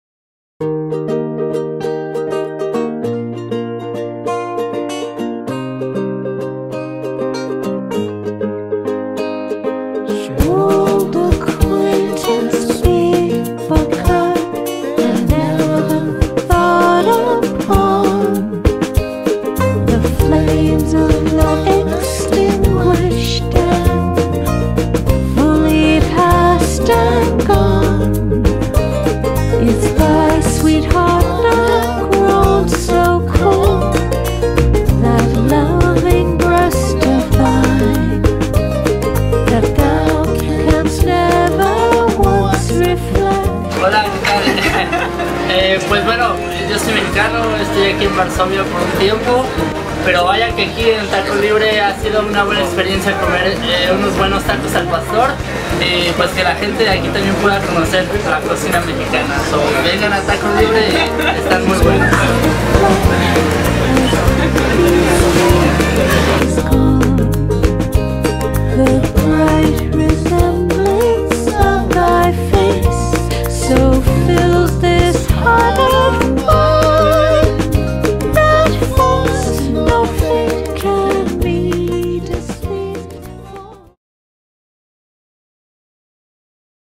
Taco libre FILMOWA WIZYTOWKA
Taco Libre pierwszy foodtruck w Warszawie serwujący meksykańskie taco.